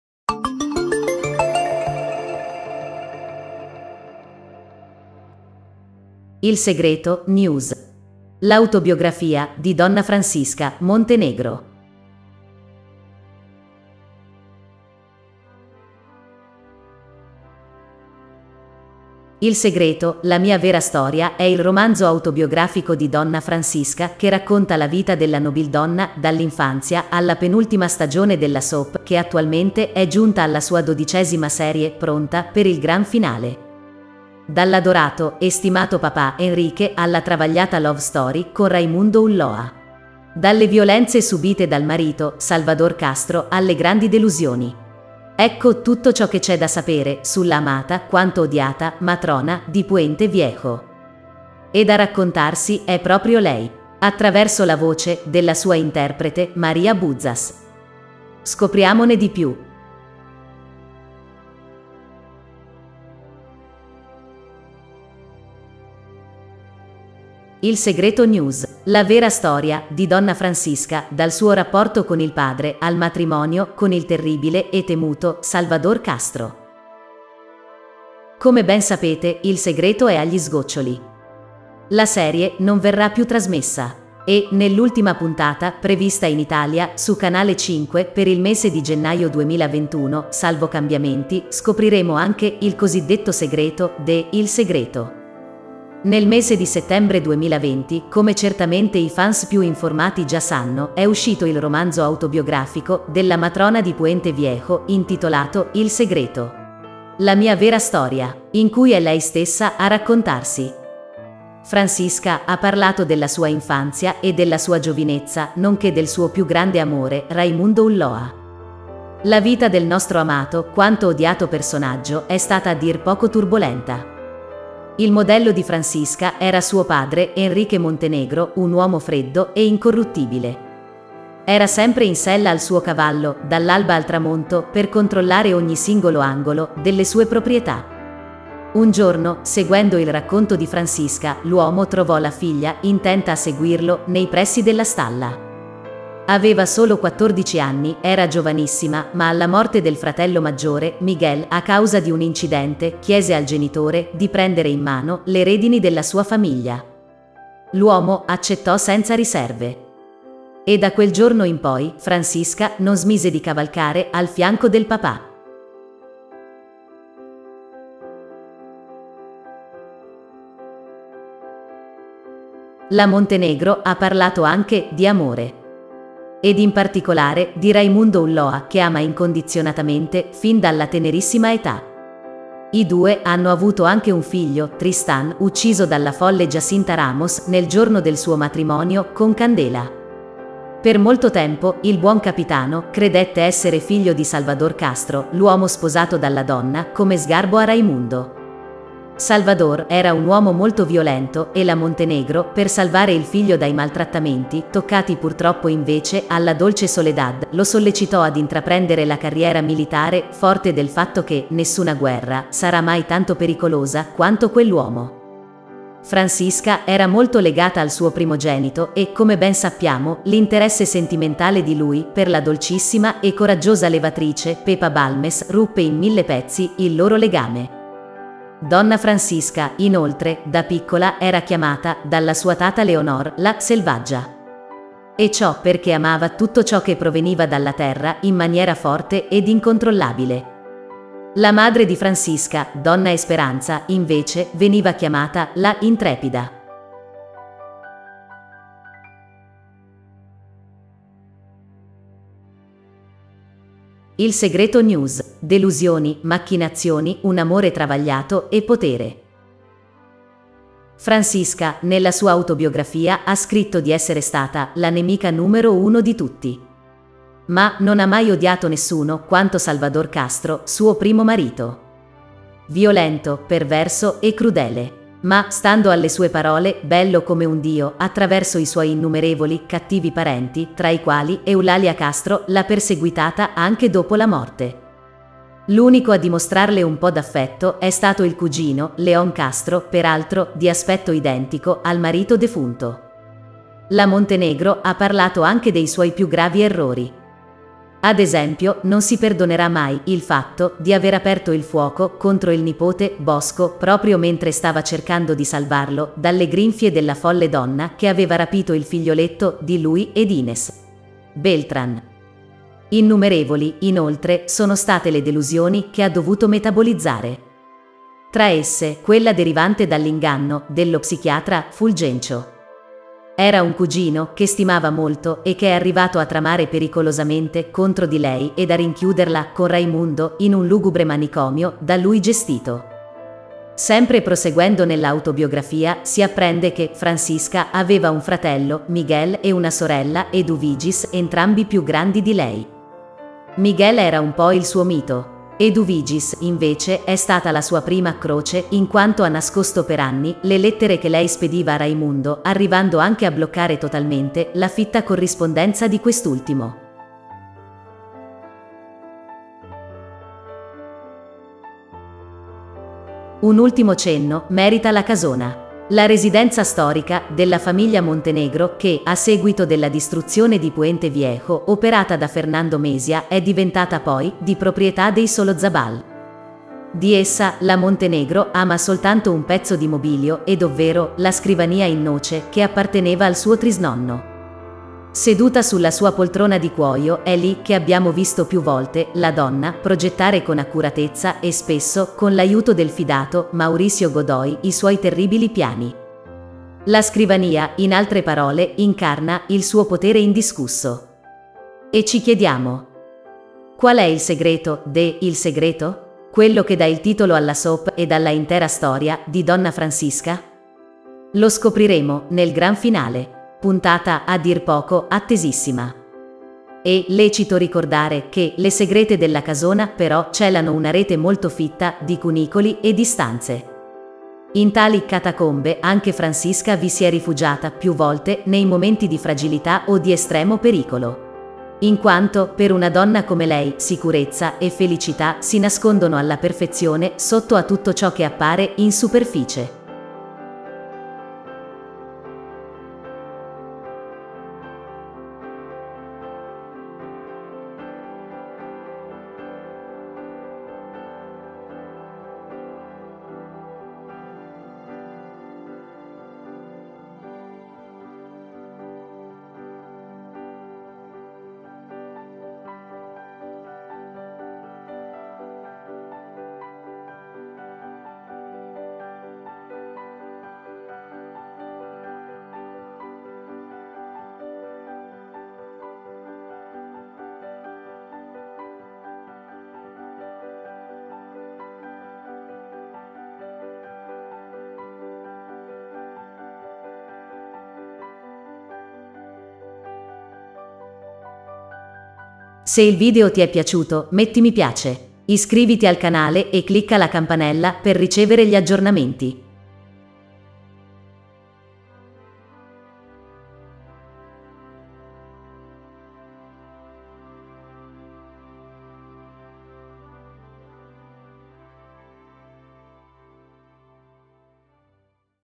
Anticipazioni Il Segreto: l’Autobiografia di Donna Francisca Montenegro!
Il Segreto, la mia vera storia è il romanzo autobiografico di Donna Francisca  che racconta la vita della nobildonna dall’infanzia alla penultima stagione della soap che, attualmente, è giunta alla sua dodicesima serie, pronta per il Gran Finale. Dall’adorato e stimato papà Enrique alla travagliata love story con Raimundo Ulloa, dalle violenze subite dal marito Salvador Castro alle grandi delusioni… ecco tutto ciò che c’è da sapere sulla amata quanto odiata matrona di Puente Viejo. Ed a raccontarsi è proprio lei, attraverso la voce della sua interprete, Maria Bouzas. Scopriamone di più!